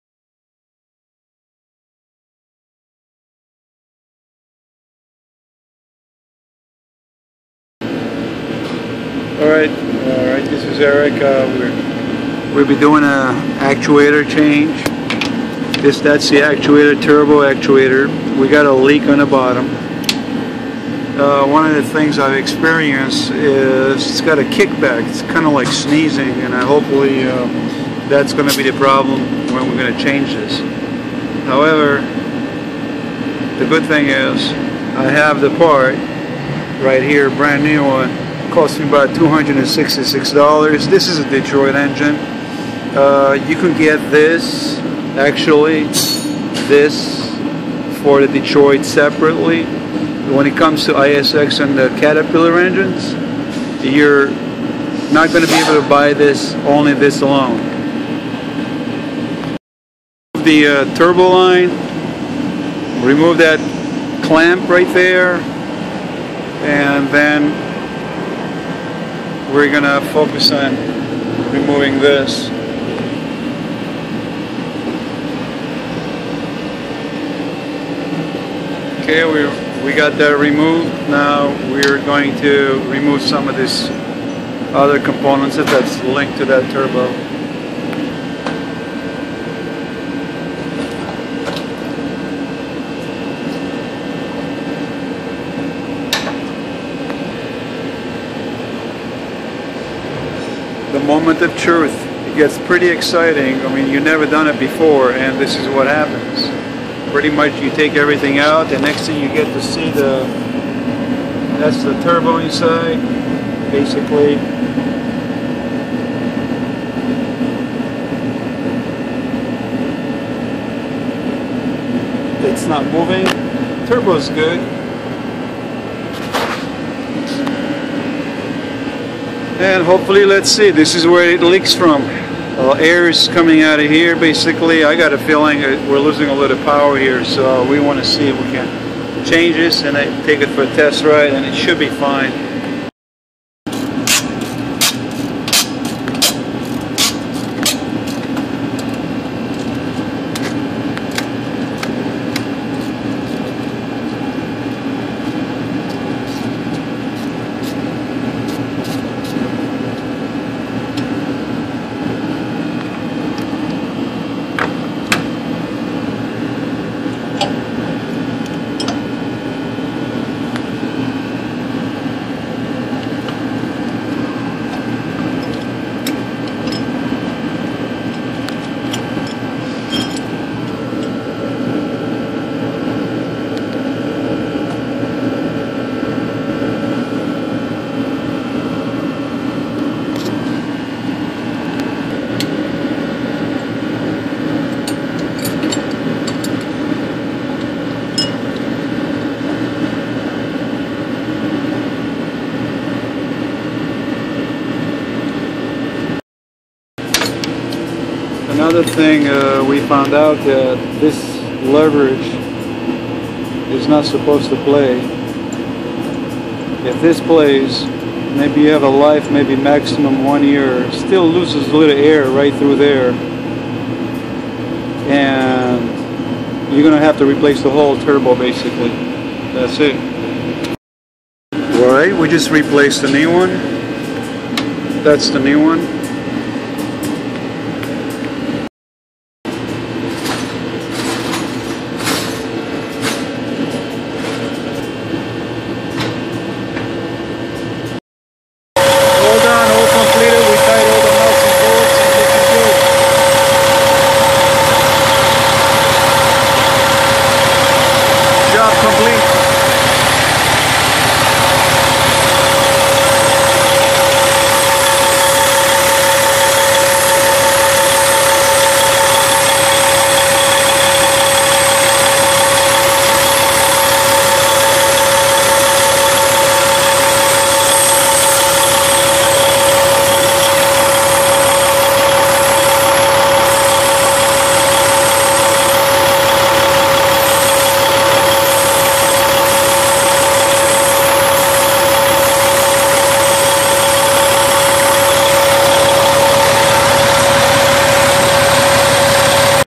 Detroit Diesel Engine turbo actuator valve damaged need replacement
I was losing power when I was climbing the hill, the turbo power was dropping under pressure,the actuator is jumping up and down and losing air out of the gasket.
It was time to change it and we did, I am glad I found the problem. the good news is that you can buy just the actuator valve, yes guys you can, it will cost you less then $300.00  You don't have to buy the turbo to fix the problem. once we did replace the valve we got back the power.